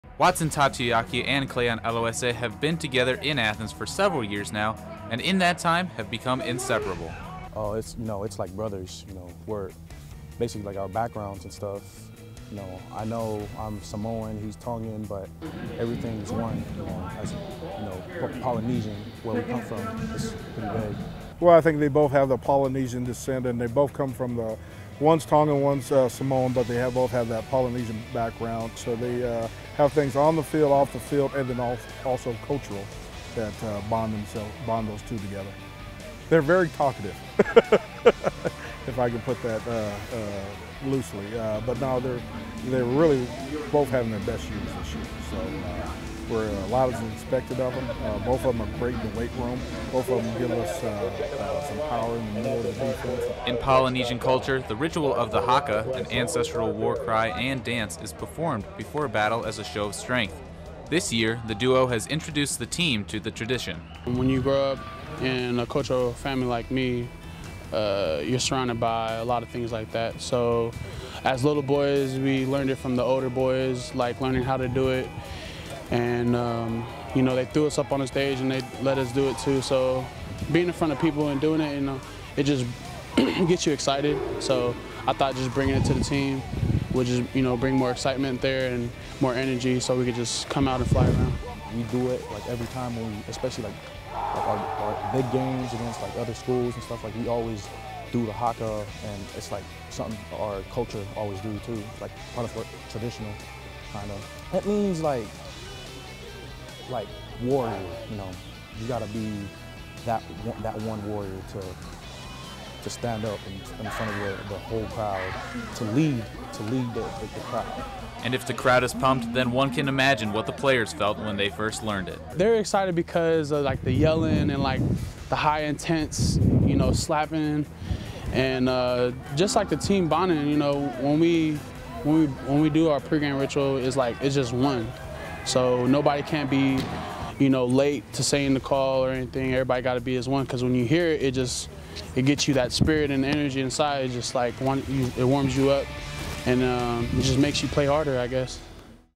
Ohio Football: The Bond of Brothers
Watson Tautuiaki and Cleon Aloese both play on the defensive line but their similarities don't end there. Both are of Polynesian descent and their connection reaches far beyond the field. Bobcats Sports Showcase reporter Sean Neidig takes a look at their special bond.